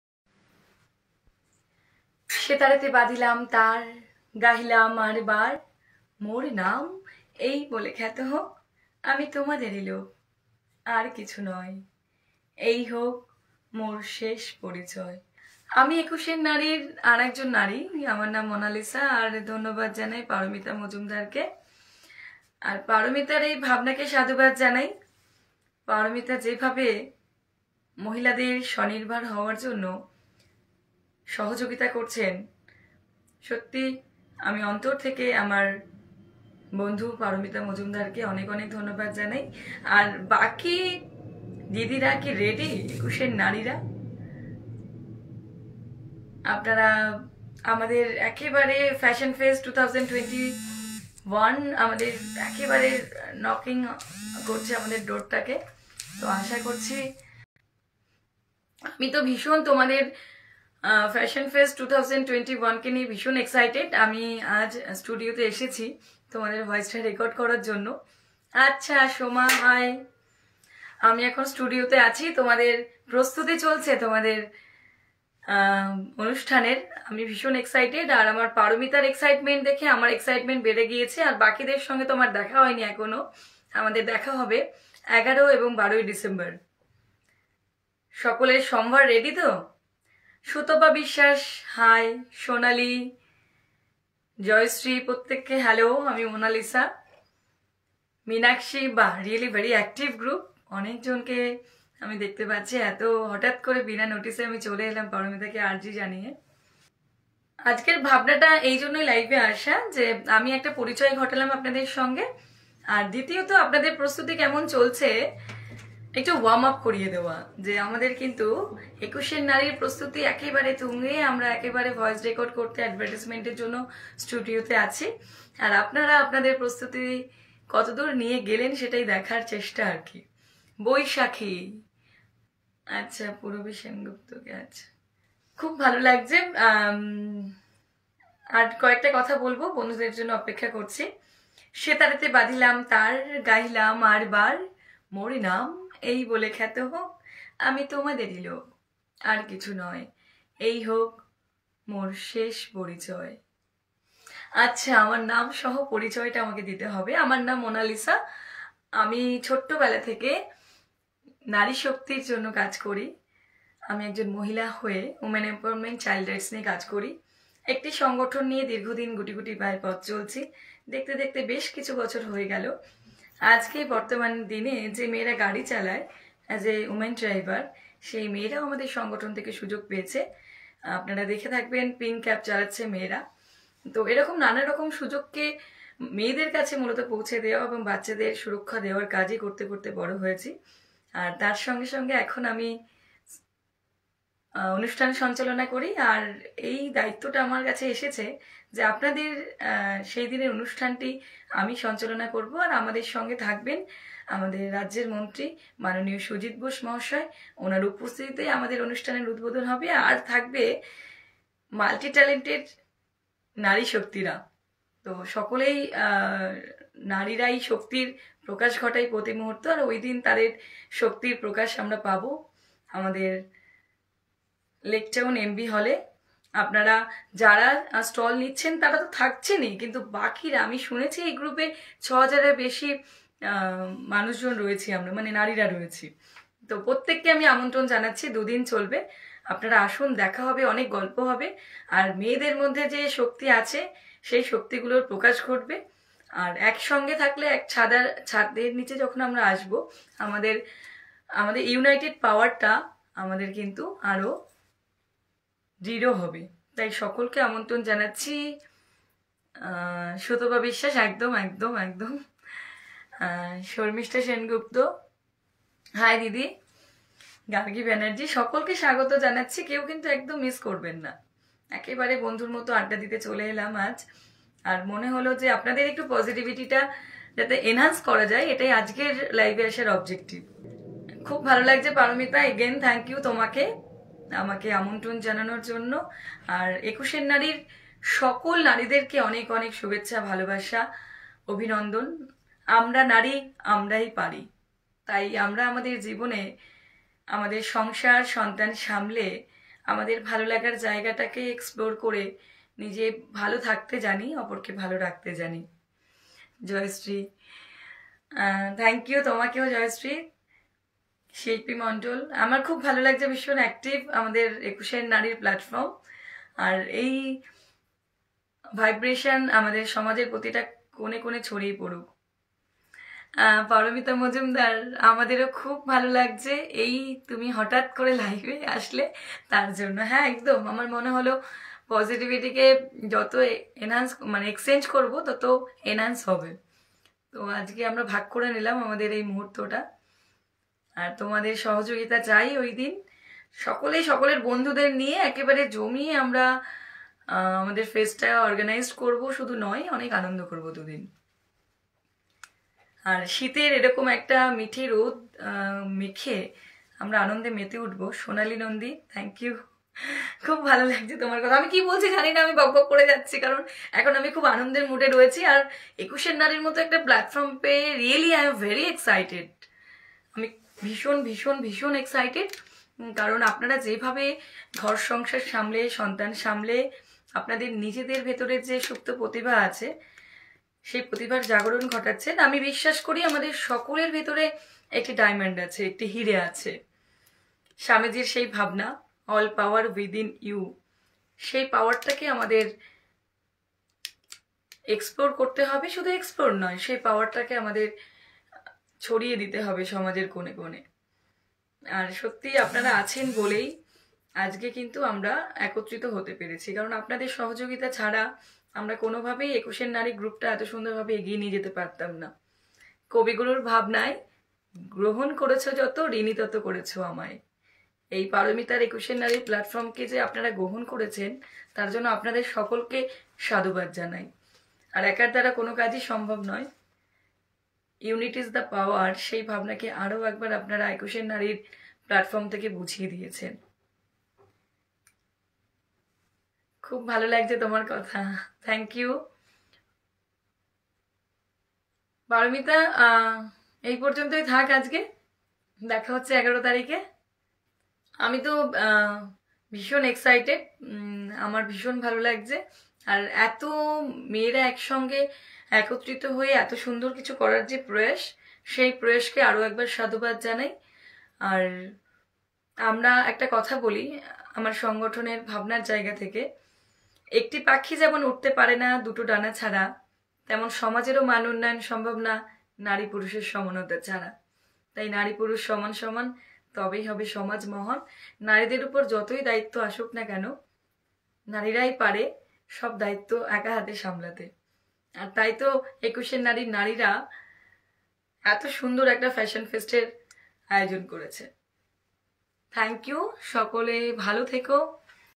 Ekusher Naree - Ekusher Naree Offline Exhibition - Fashion Fest'21
Ekusher Naree (Lady on 21st) is female Facebook buying & selling group.